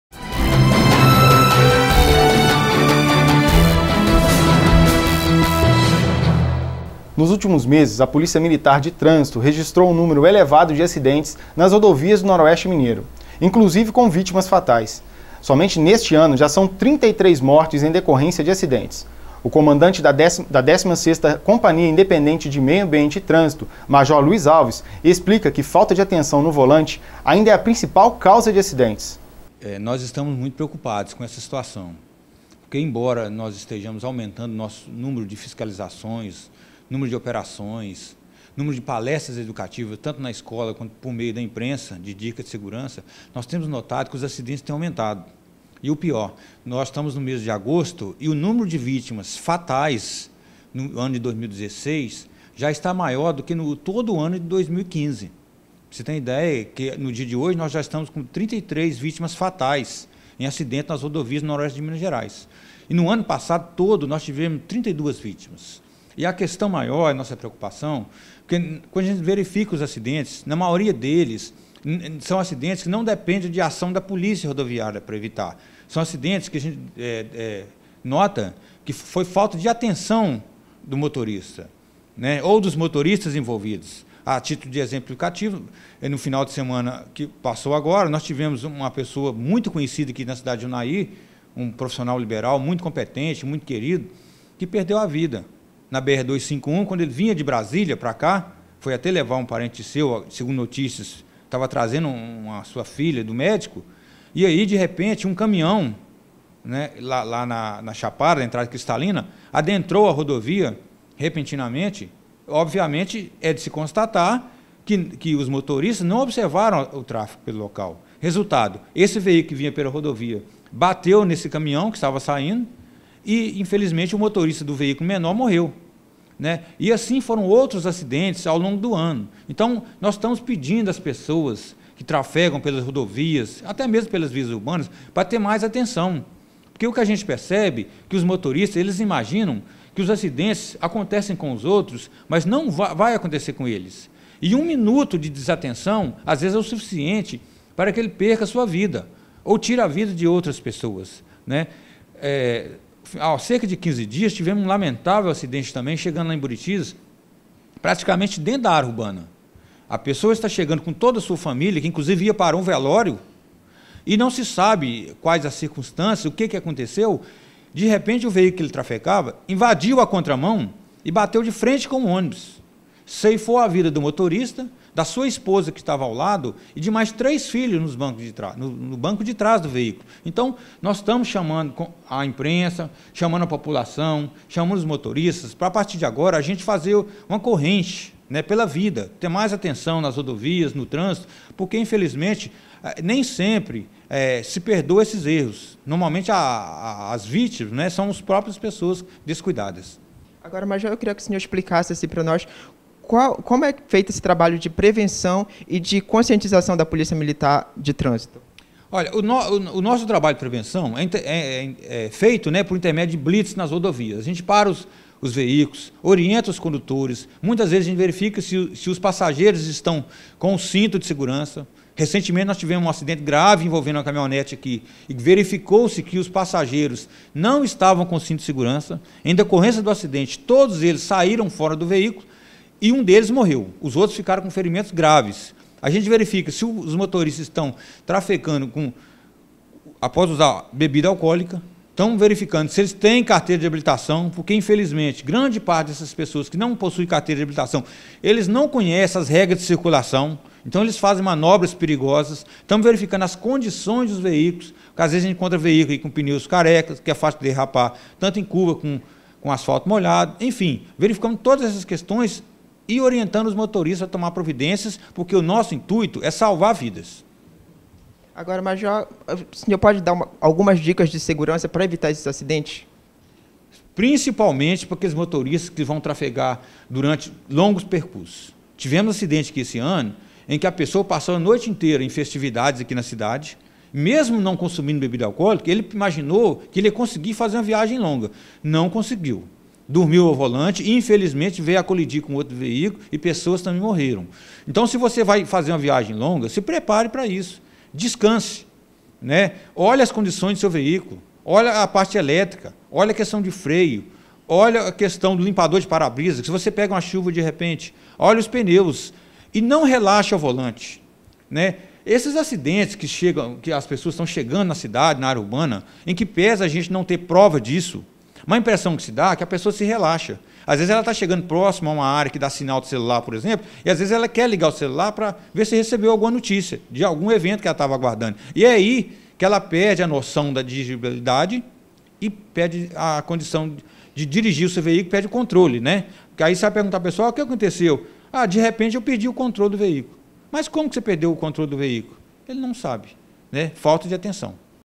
33 vitimas fatais em decorrência a acidentes de transito no Noroeste Mineiro
Nos últimos meses, a polícia militar de trânsito registrou um número elevado de acidentes nas rodovias do noroeste mineiro, inclusive com vítimas fatais. Somente neste ano já são 33 mortes em decorrência de acidentes. O comandante da décima sexta companhia independente de meio ambiente e trânsito, major Luiz Alves, explica que falta de atenção no volante ainda é a principal causa de acidentes.